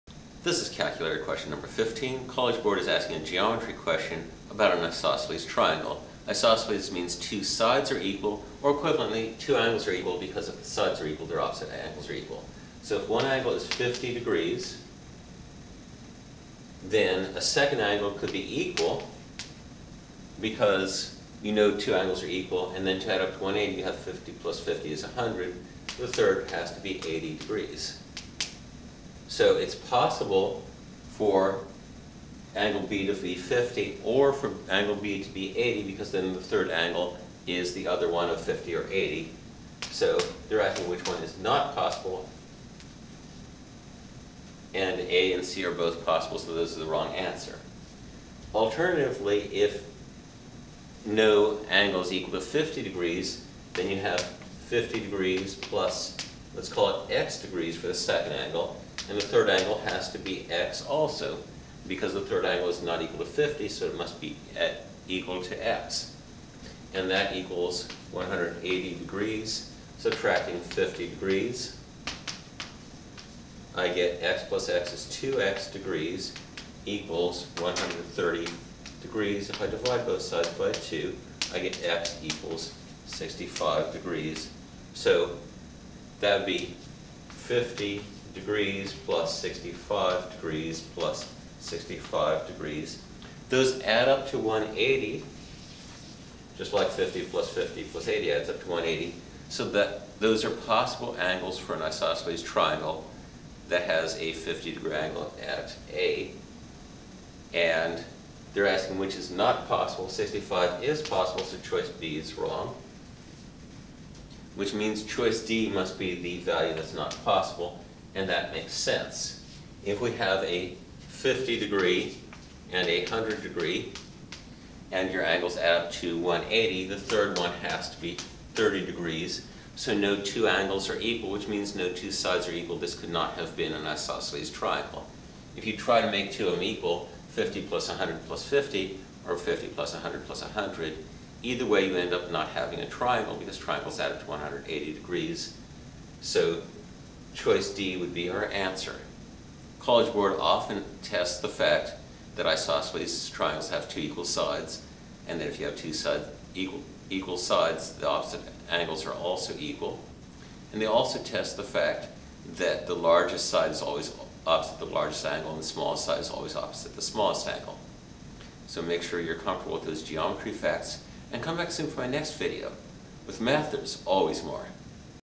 SAT Oct 2019: Triangles - question #4-15
In triangle ABC, the measure of angle A is 50 degrees. If triangle ABC is isosceles, which of the following is NOT a possible measure of angle B?